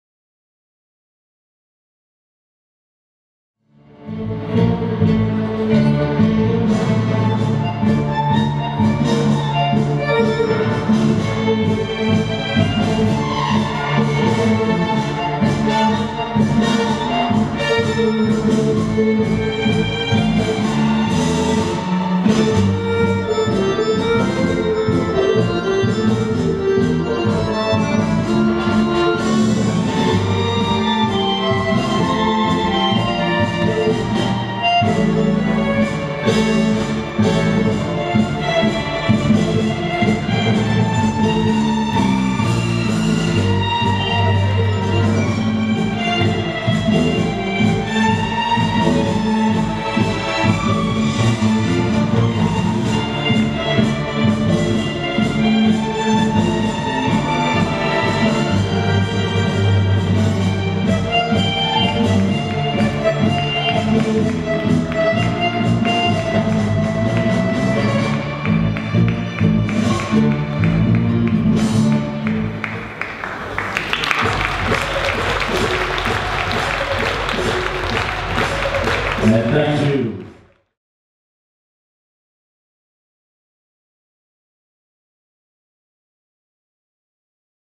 Dariusz Mycka - Madara Freiberga, POL | Diamond Cup Antwerp 2018 - WO STD - solo T
Diamond Cup 2018, Antwerp/Belgium - WDSF World Open Standard

Final Results:
1. Evgeny Nikitin - Anastasia Miliutina      RUS 281
2. Anton Besedin - Ekaterina Strelkova     RUS 326
3. Matteo Cicchitti - Simona Brecikova    SVK 139
4. Dariusz Mycka - Madara Freiberga       POL 219
5. Daniil Ulanov - Kateryna Isakovych      CYP         168
6. Winson Tam - Anastasia Novikova      CAN         113